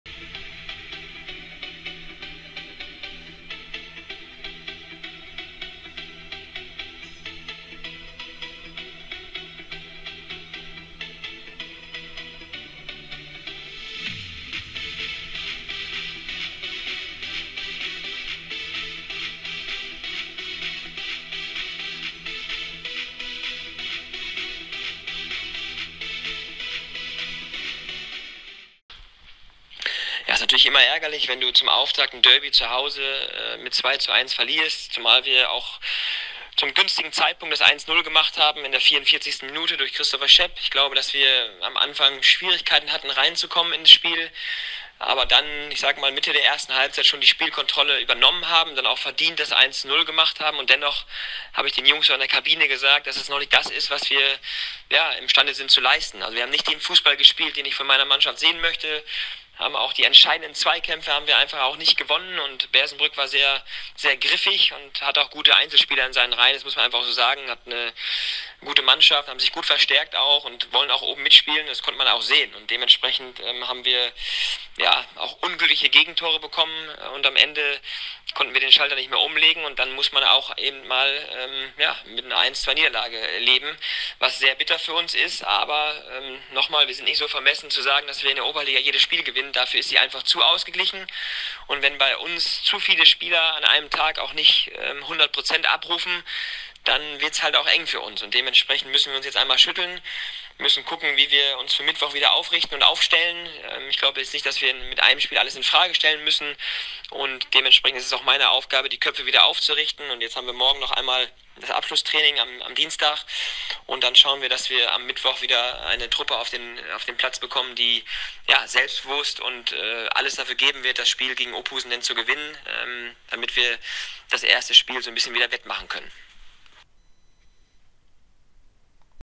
21 08 08 Fazit TuS BW Lohne-TuS Bersenbrück
Fazit BW Lohne Trainer Henning Riesßelmann